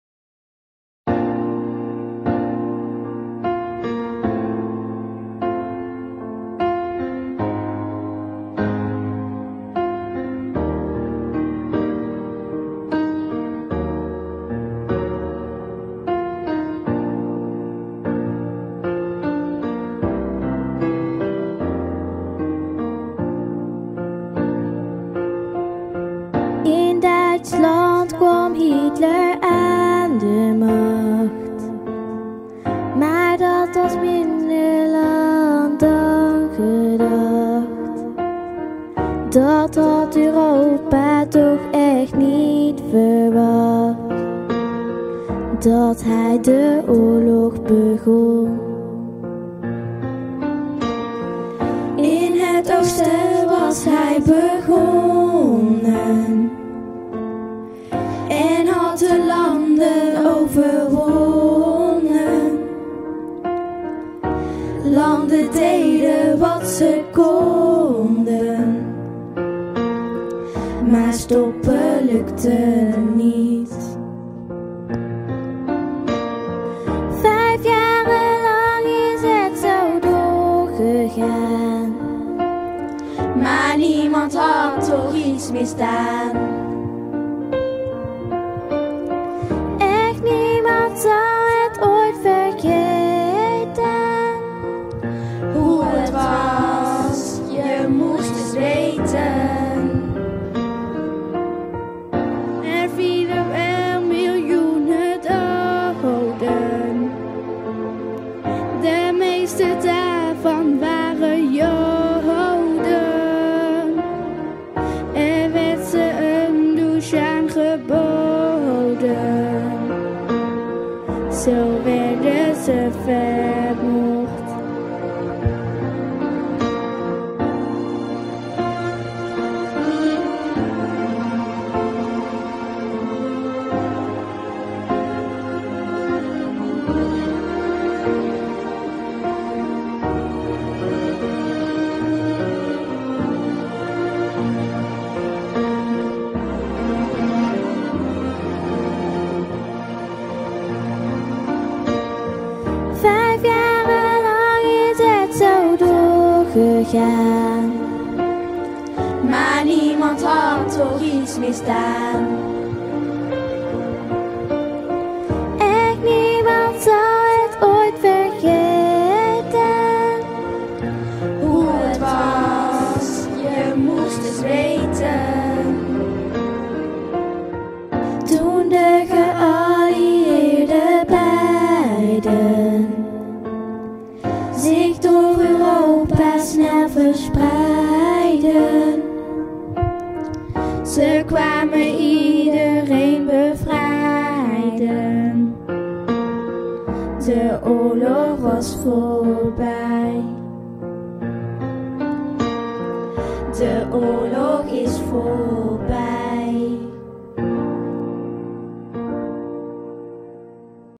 Liedje over 2e Wereldoorlog bs Klinkers
Kinderen van groep 7/8 hebben zelf een liedje geschreven over de Tweede Wereldoorlog. Kernconcept Macht & regels. Februari 2013..